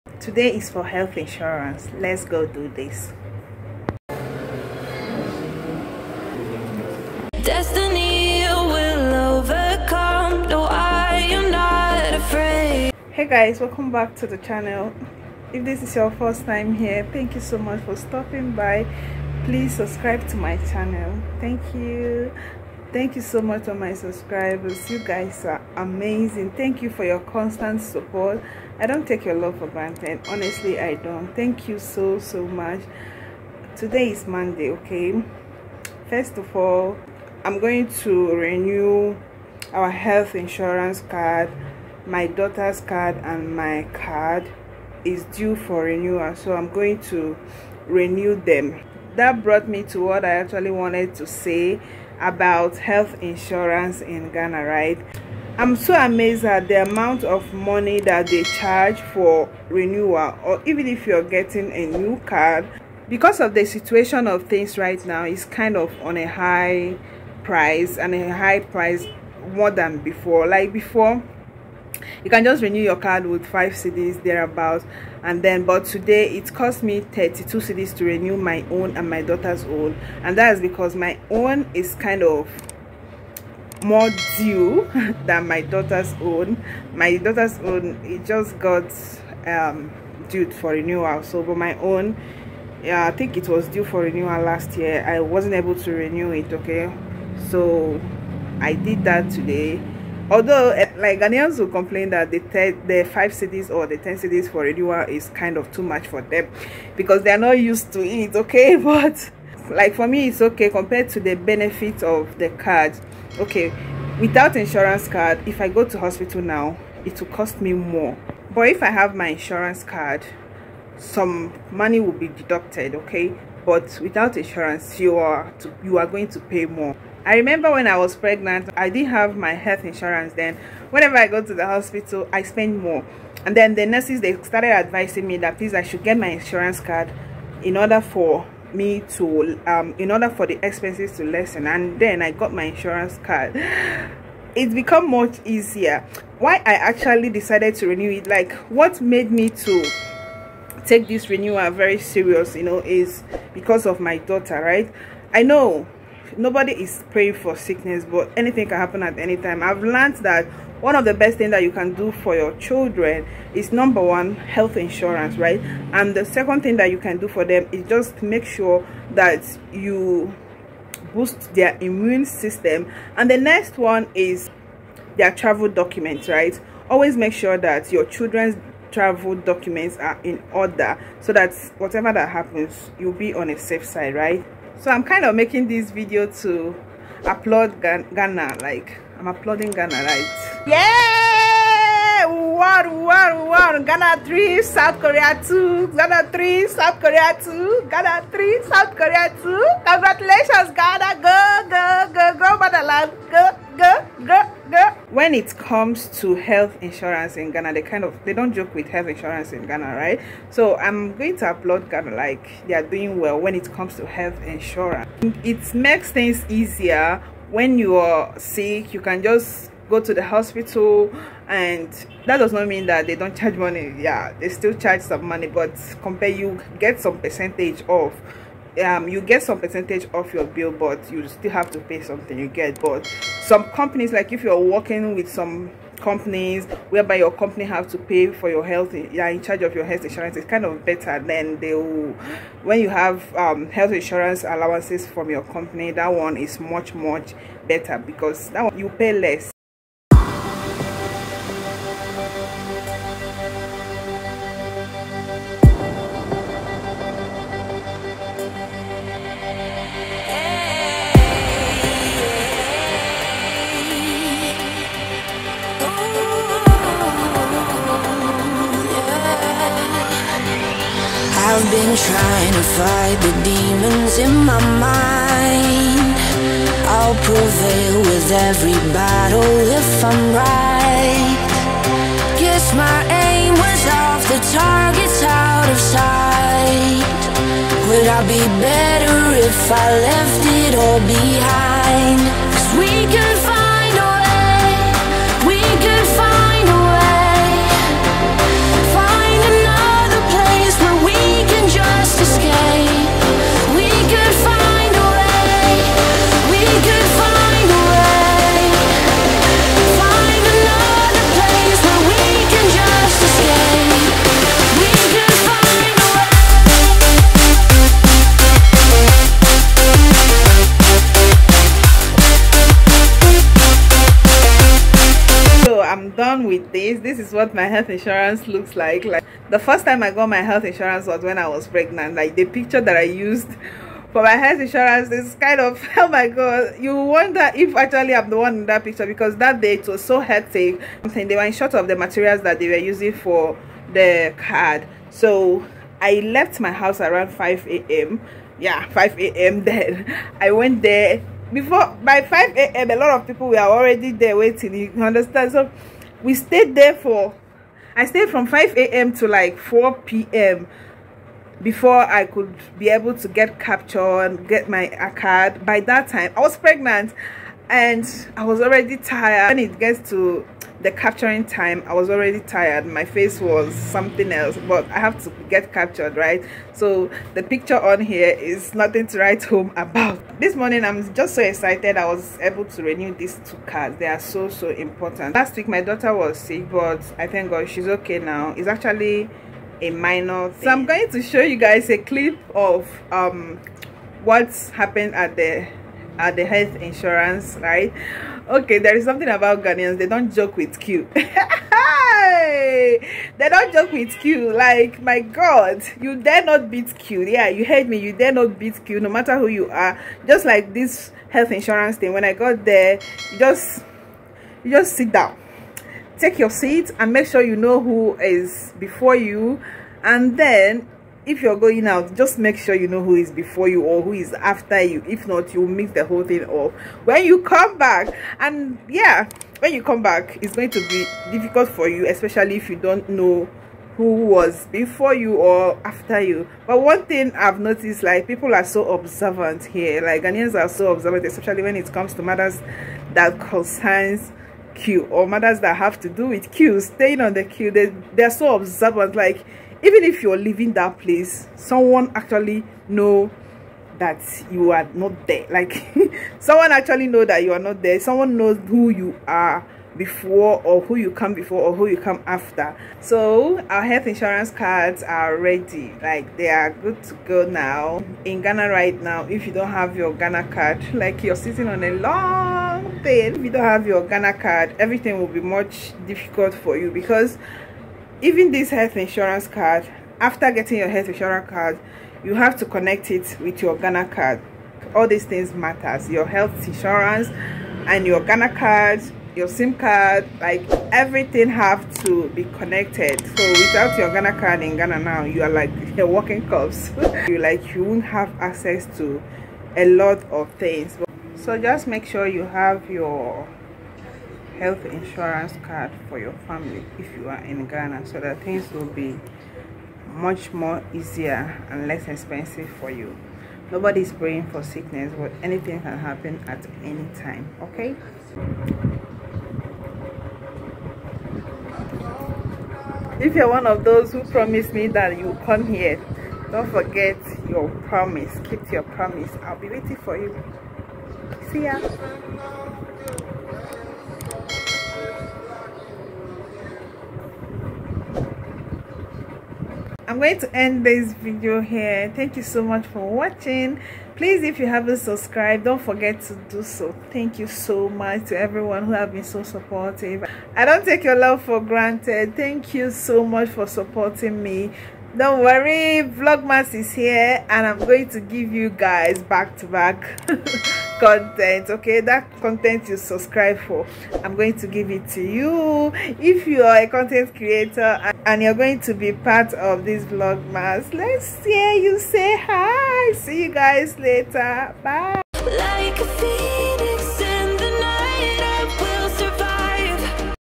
Excitement In Ghana 🇬🇭 As They Won South Korea Today | Getting Our Health Insurance In Ghana 🇬🇭 🇳🇬
Thank you so much for your continuous support.
Today is a special day in Ghana 🇬🇭 as Ghana won South Korea.
We're excited and I got my health insurance.
Why you need health insurance in Ghana 🇬🇭

Super Shocked To See How Ghanaians 🇬🇭

 Exploring Ghana 🇬🇭 Fabric Haul in the biggest market in Ghana, Makola Market